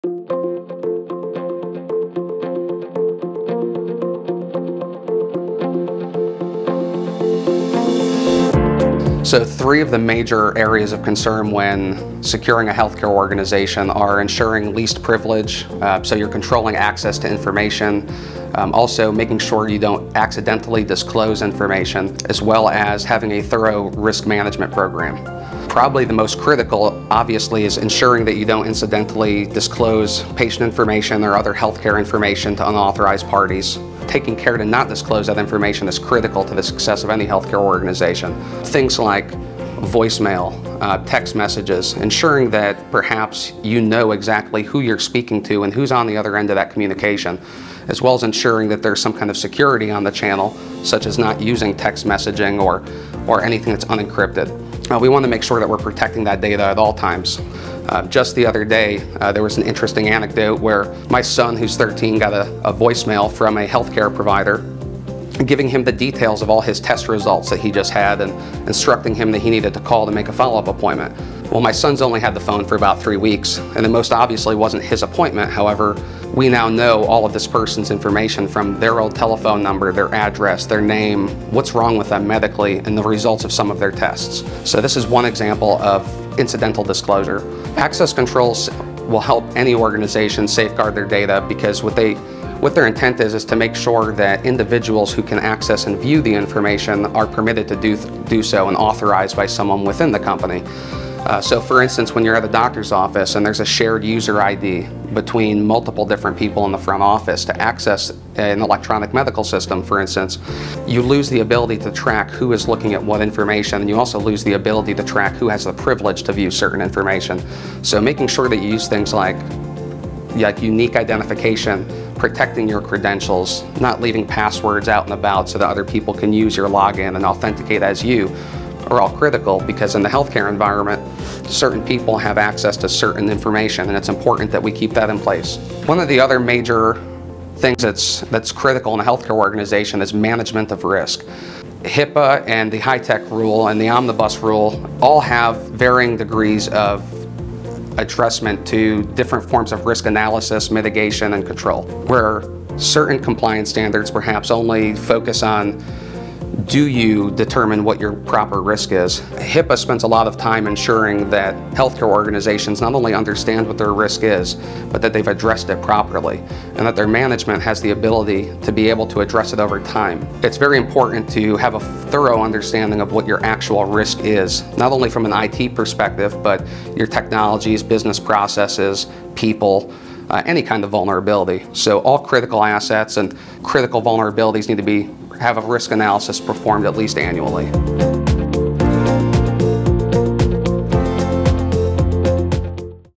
Healthcare Data Security Best Practices | ControlScan
Learn the healthcare data security best practices that will keep protected health information (PHI) and payment card data secure and your organization compliant with PCI DSS and HIPAA. Expert healthcare data security advice from Marc Punzirudu, @punzirudu.

Security Best Practices for Healthcare Organizations:
Three of the major areas of concern when securing a healthcare organization are ensuring least privilege, so you're controlling access to information; also making sure you don't accidentally disclose information; as well as having a thorough risk management program. As far as those 3 things are concerned, probably the most critical obviously is ensuring that you don't incidentally disclose patient information or other healthcare information to unauthorized parties.

Taking care to not disclose that information is critical to the success of any healthcare organization. Things like voice mail, text messages, ensuring that perhaps you know exactly who you're speaking to and who's on the other end of that communication, and well as ensuring that there's some kind of security in the channel, such as not using text messaging or anything that's unencrypted. We want to make sure that we're protecting that data at all times.

Just the other day there was an interesting anecdote where my son who's 13 got a voice mail from a healthcare provider giving him the details of all his test results that he just had and instructing him that he needed to call to make a follow-up appointment. Well my son's only had the phone for about 3 weeks and it most obviously wasn't his appointment; however we now know all of this person's information from their old telephone number, their address, their name, what's wrong with them medically, and the results of some of their tests. This is one example of incidental disclosure.

Access controls will help any organization safeguard their data, because what their intent is, is to make sure that individuals who can access and view the information are permitted to do so and authorized by someone within the company. For instance when you're at a doctor's office and there's a shared user ID between multiple different people in the front office to access an electrical medical system for instance, you lose the ability to track who is looking at what information, and you also lose the ability to track who has the privilege to view certain information. Making sure that you use things like unique identification, protecting your credentials, not leaving passwords out and about so that other people can use your login and authenticate as you are all critical, because in the healthcare environment certain people have access to certain information and it's important that we keep that in place.

One of the other major things that's critical in a healthcare organization is management of risk. HIPAA and the High Tech Rule and the Omnibus Rule all have varying degrees of addressment to different forms of risk analysis, mitigation and control. Where certain compliance standards perhaps only focus on do you determine what your proper risk is, HIPAA spends a lot of time ensuring that healthcare organizations not only understand what their risk is but that they've addressed it properly, and that their management has the ability to be able to address it over time. It's very important to have a thorough understanding of what your actual risk is, not only from an AT perspective, but your technologies, business processes, people, any kind of vulnerability. All critical assets and critical vulnerabilities need to be have a risk analysis performed at least annually.